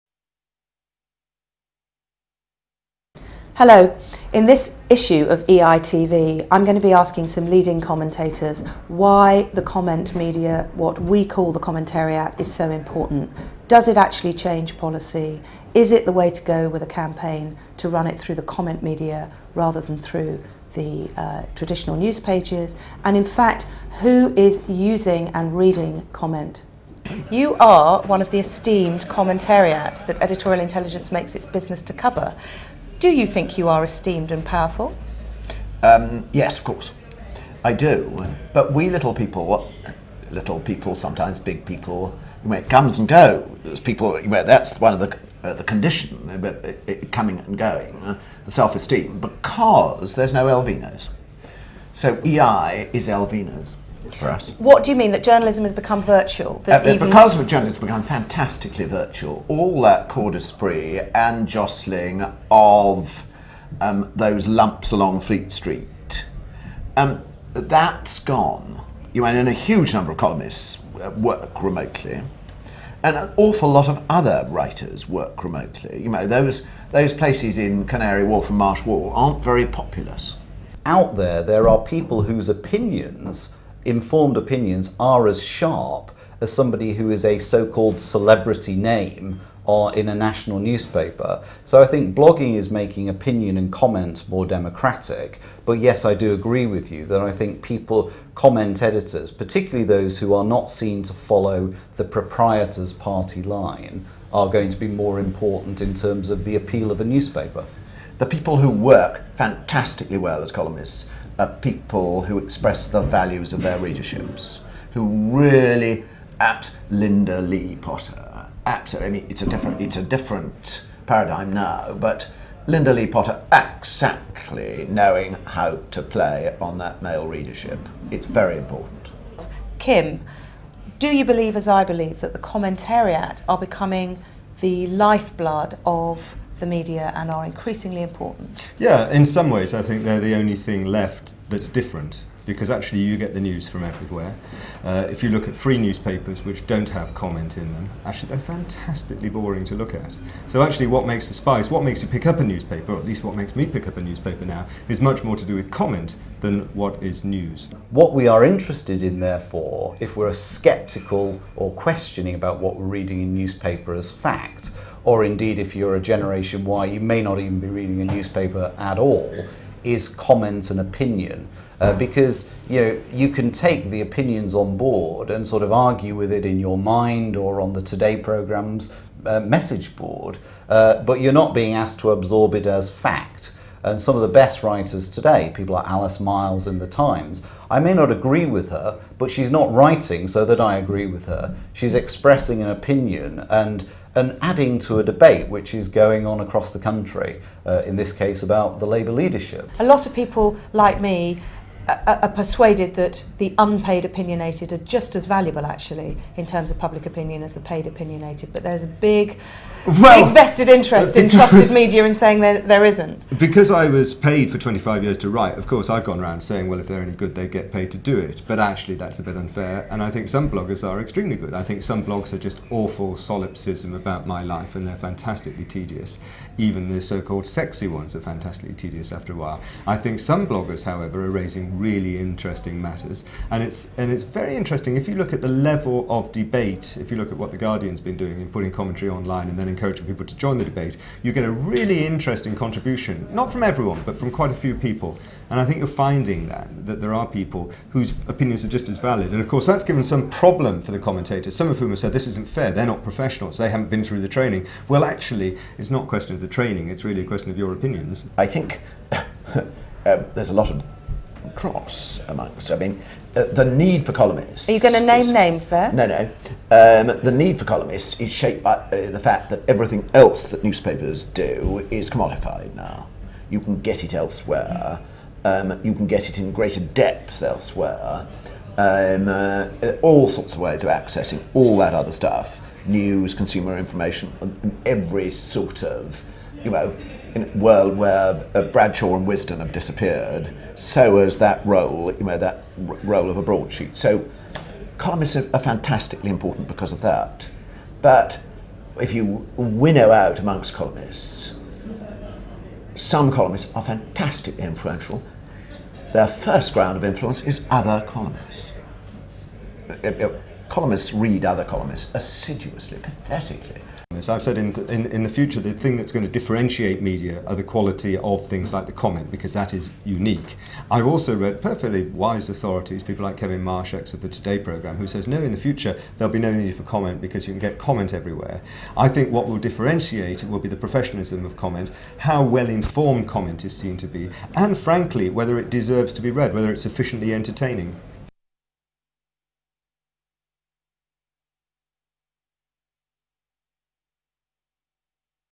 Commentariat Comment with Peter York, Colin Byrne, Kim Fletc
Chief Executive Julia Hobsbawm with key commentators and opinion makers.